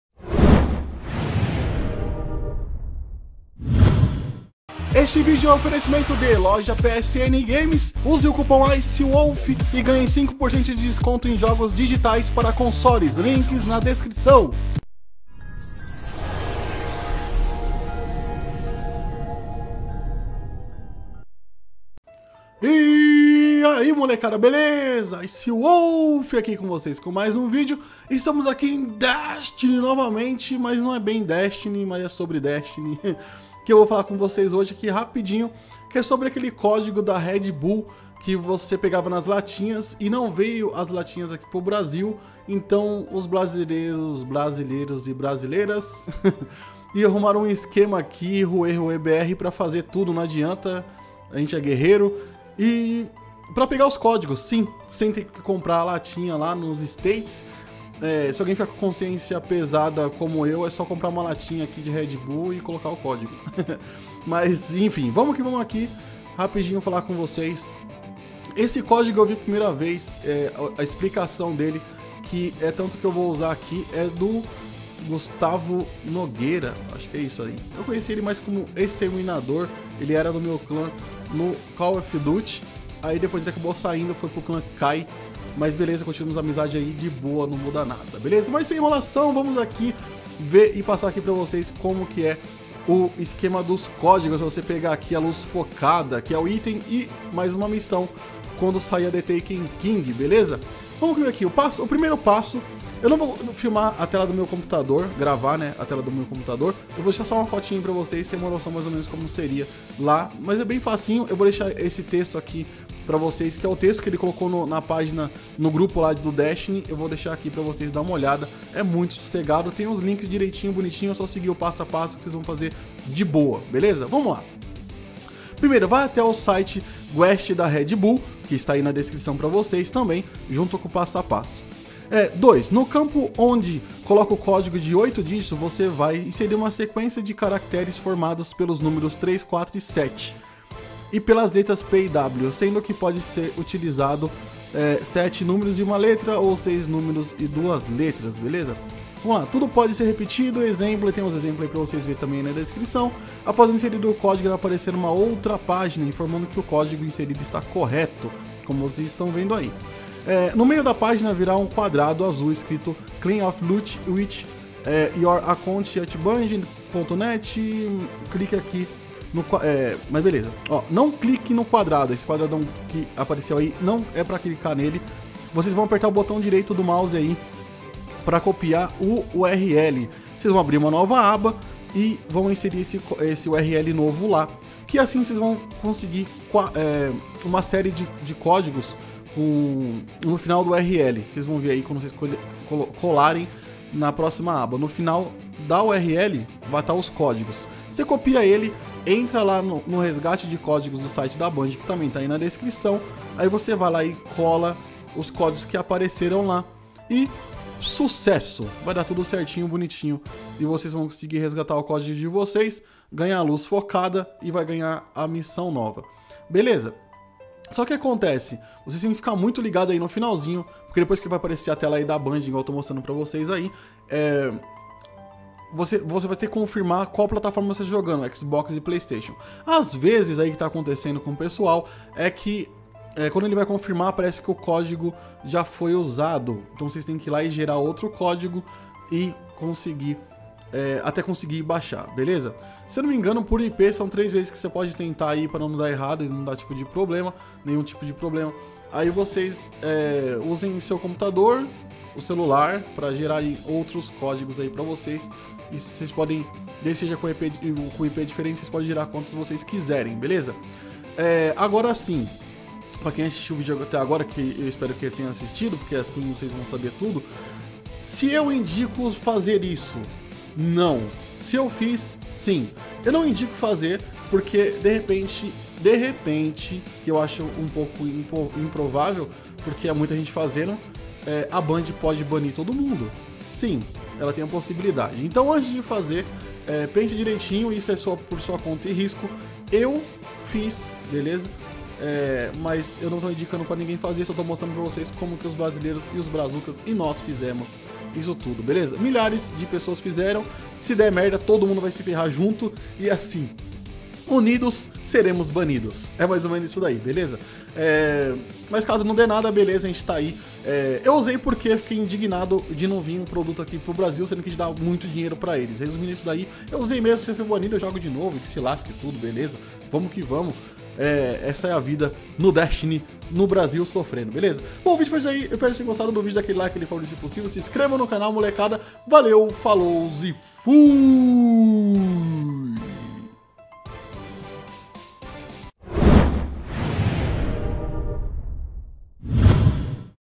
DESTINY - LUZ FOCADA ILIMITADA REDBULL
1 - Vá até o site da quest do red bull
2 - No campo onde coloca o código de 8 dígitos, você irá inserir uma sequência de caracteres formados pelos números 3, 4 e 7, e pelas letras P e W, sendo que pode ser utilizado 7 números e 1 letra ou 6 números e 2 letras. TUDO PODE SER REPETIDO.
EXEMPLO: 3333333P 4444444W 334477PP 774433WW WW334477 W347347P P743743W PP333444 WW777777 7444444P 3777774W
3 - Após inserir o código irá aparecer outra página informando que o código inserido está correto. No meio da página terá um quadrado azul escrito CLAIM THIS LOOT WITH YOUR ACCOUNT AT BUNGIE.NET.
NÃO CLIQUE NO QUADRADO PARA IR PARA O SITE DA BUNGIE.
COPIE O ENDEREÇO DO LINK DESSE QUADRADO AZUL PRIMEIRO.
Para fazer isso é só clicar com o botão direito do mouse e ir na opção COPIAR ENDEREÇO DO LINK.
Depois de ter copiado o link, aí sim você poderá ir para o site da Bungie.
4 - No final do link copiado existirá uma sequencia de caracteres de 14 dígitos como no exemplo acima. COPIE ESSES 14 DÍGITOS
AAA-BBB-CCC-DDDDD
5 - No site da Bungie onde pede para inserir o código, o campo informado pede que o código inserido tenha 9 dígitos.
6 - COLE O CÓDIGO DE 14 DÍGITOS NO CAMPO ONDE PEDE O CÓDIGO DE 9 DÍGITOS.
7 - Aproveite o seu item no jogo.
8 - Caso queira refazer o processo, o site do red bull limita a 5 códigos por IP, por isso após a inserção dos 5 códigos será necessário alterar o IP da sua internet.
9 - Para alterar o IP da sua internet utilize programas de VPN, como o ghost VPN ou páginas de Proxy.